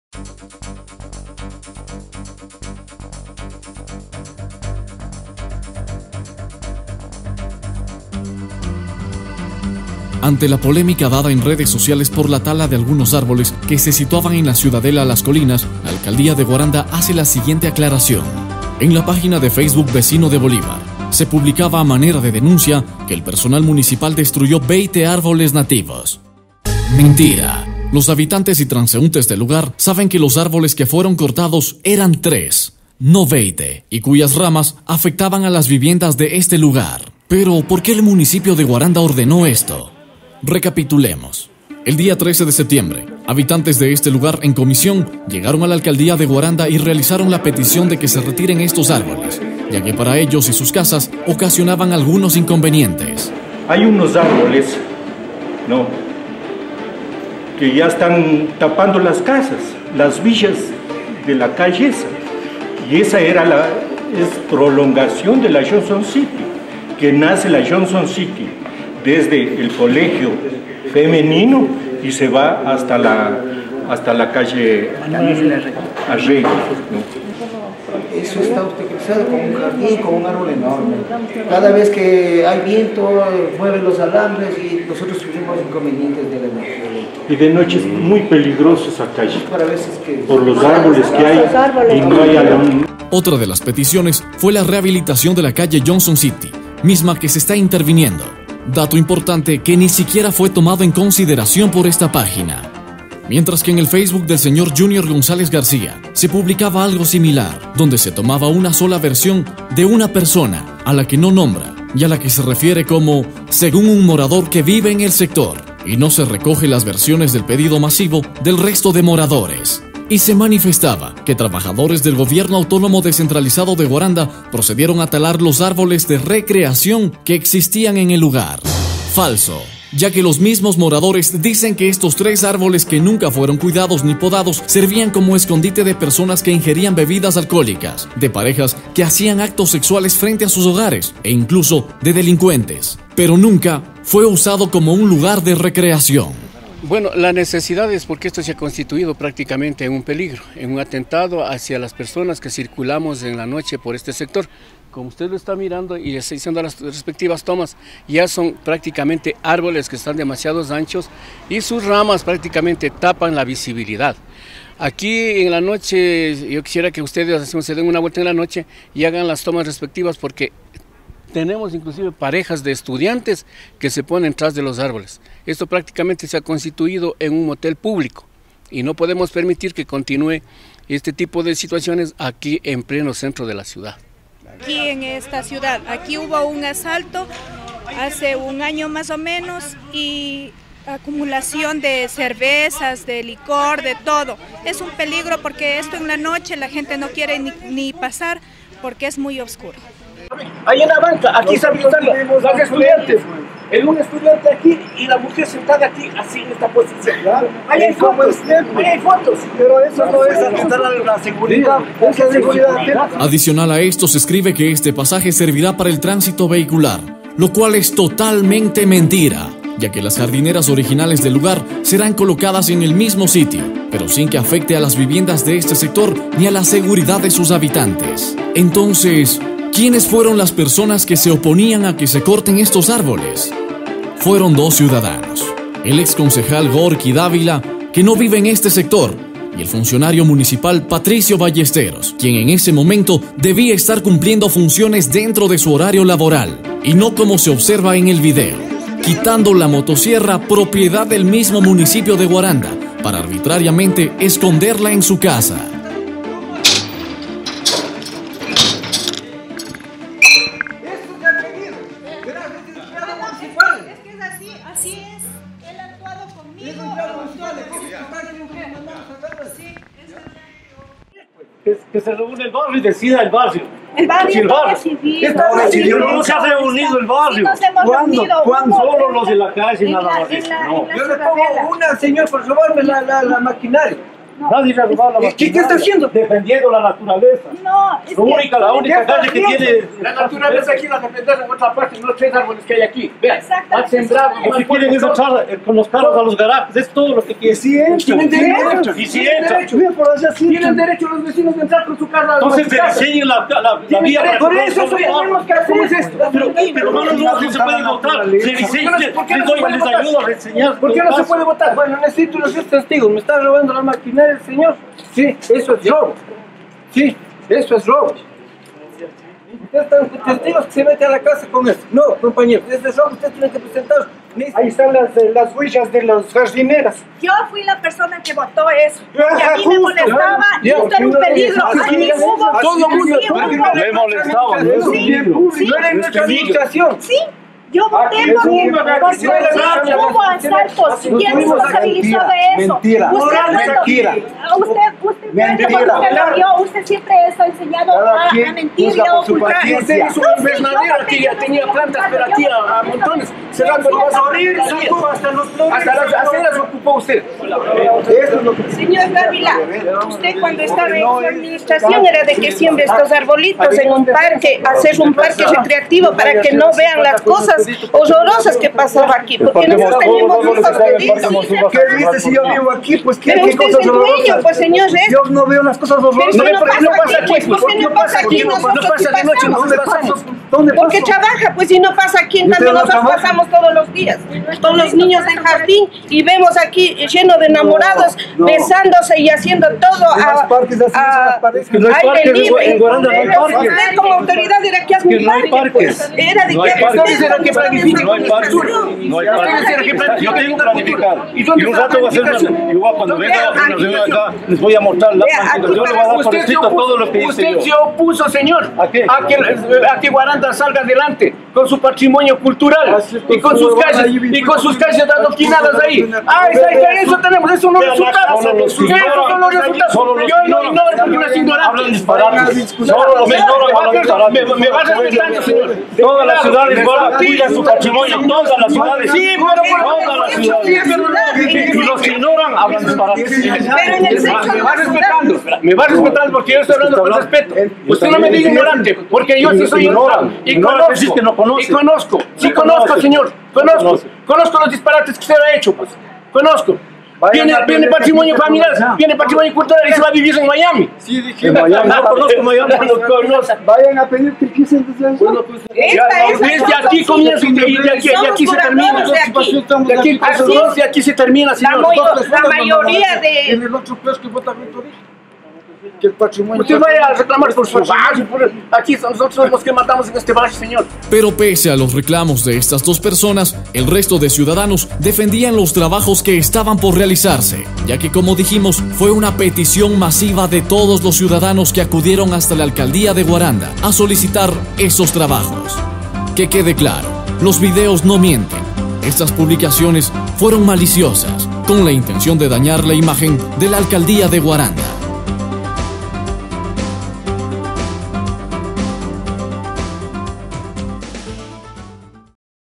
Municipio de Guaranda da la versión oficial del porque se cortaron los árboles
en el pasaje de la calle Johnson City de la ciudad de Guaranda.